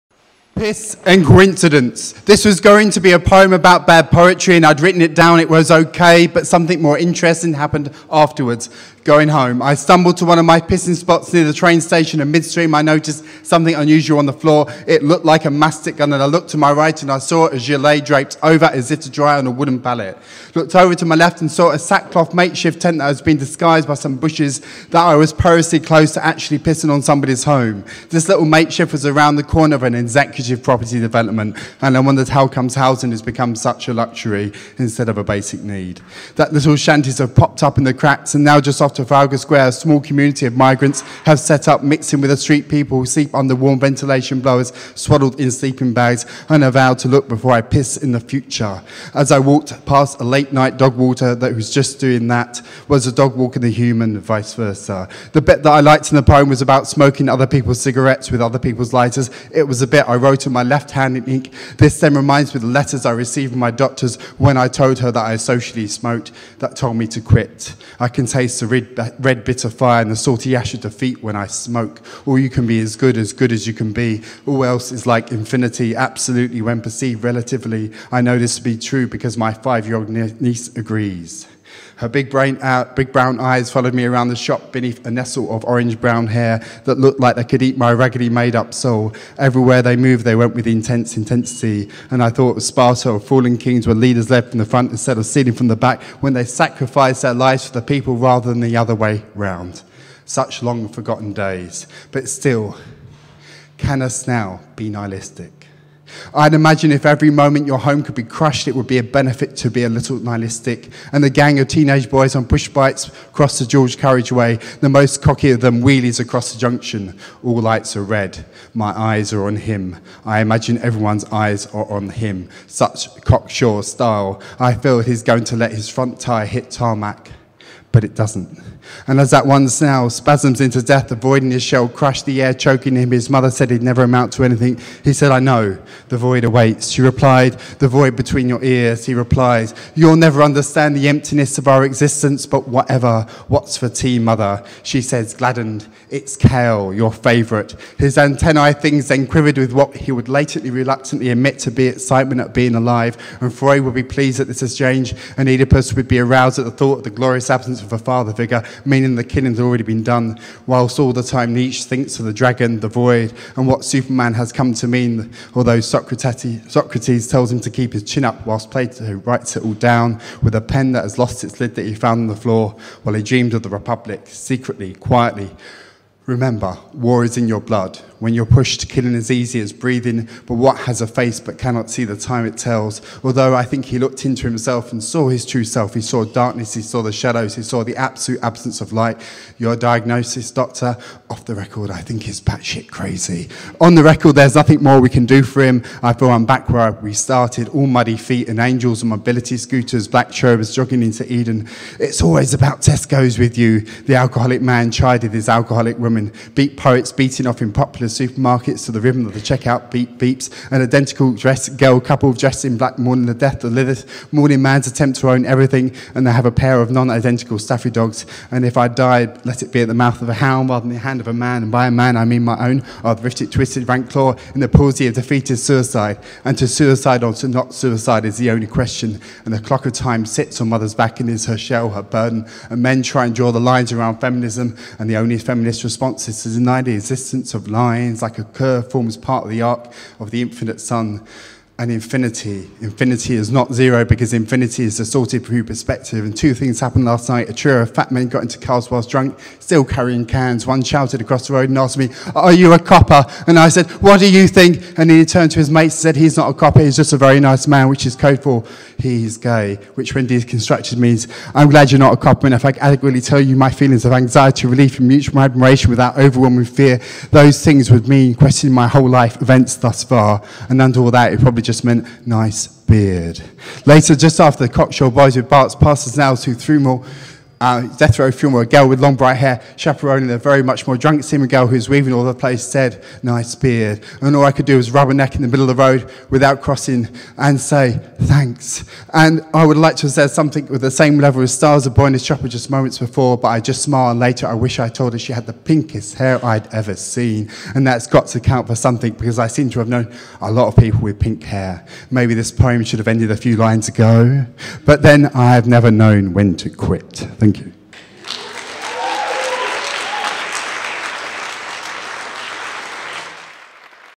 Word Up | Tom Sinclair - Piss and coincidence
Word Up is a spoken word night on the first Monday of every month at Harlesden Picture Palace.
Check us out on facebook or Twitter @WordUpLDN

Audio / Edit By WordUp